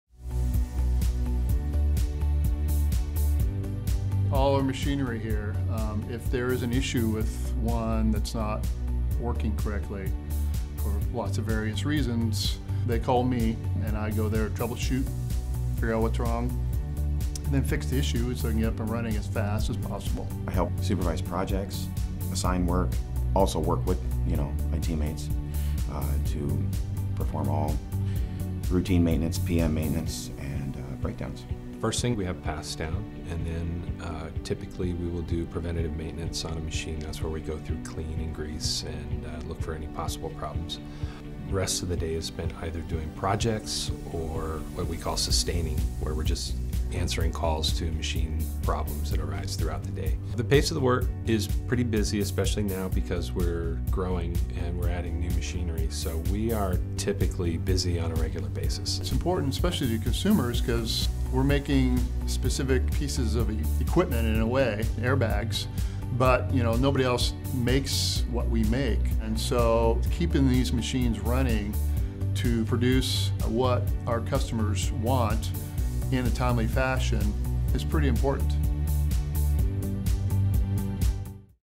Explore a Career: Maintenance at Nike Air Manufacturing Innovation
Our team of skilled industrial maintenance employees maintain and repair our specialized plastic manufacturing equipment used to make Nike’s most advanced footwear components. Maintenance technicians leverage a variety of mechanical, electrical, electronic, hydraulic and pneumatic expertise to keep the machines running smoothly and efficiently.

Interested in stretching your maintenance expertise? Find your fit and discover job openings